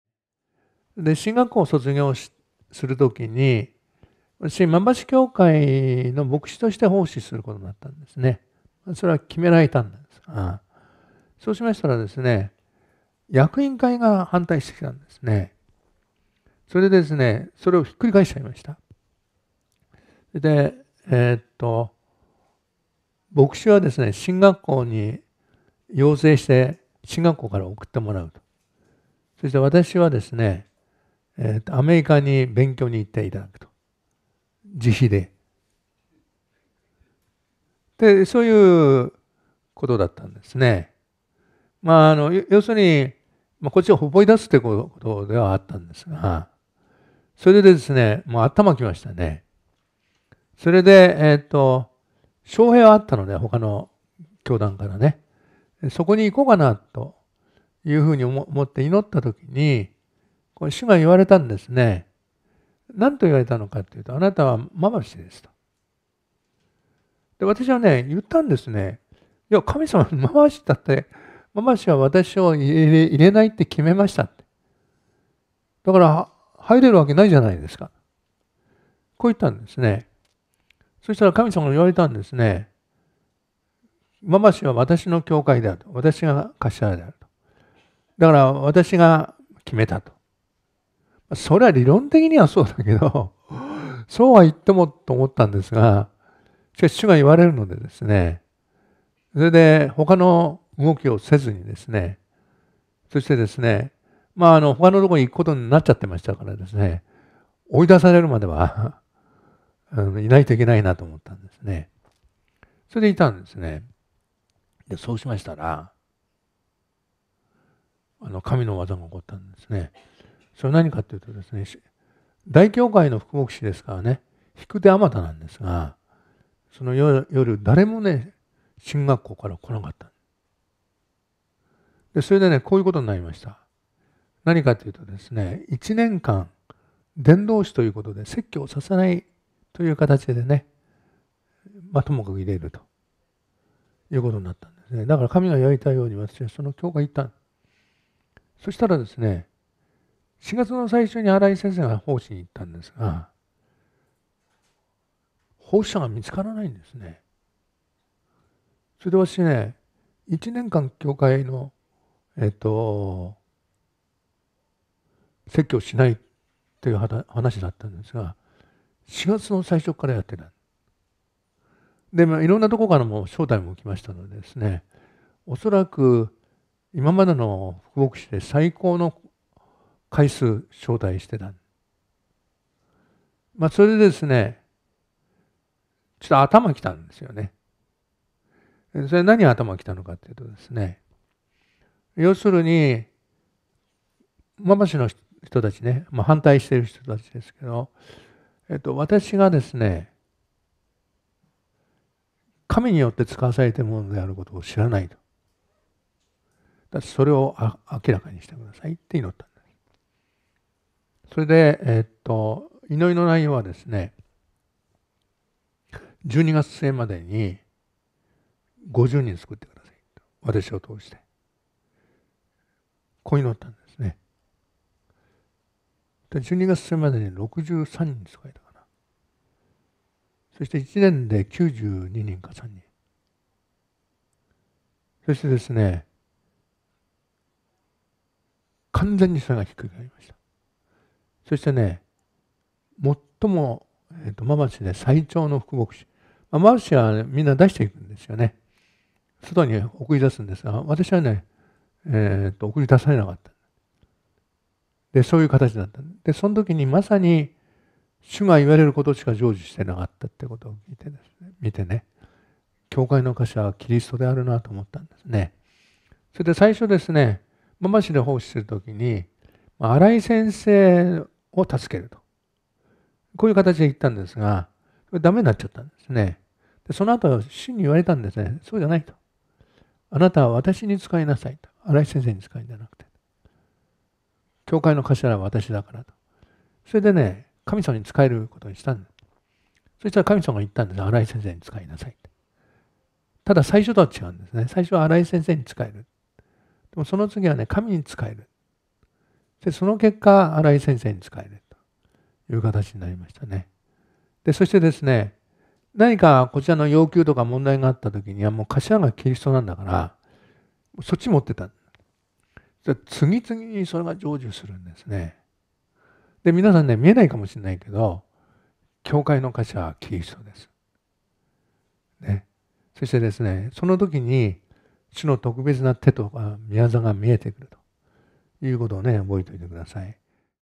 奉仕人生のスタートで神に教えられた「教会の頭はキリスト」という出来事【パウロ秋元のありえない話 150】
パウロ秋元のありえない話 150
奉仕人生のスタートで神に教えられた「教会の頭はキリスト」という出来事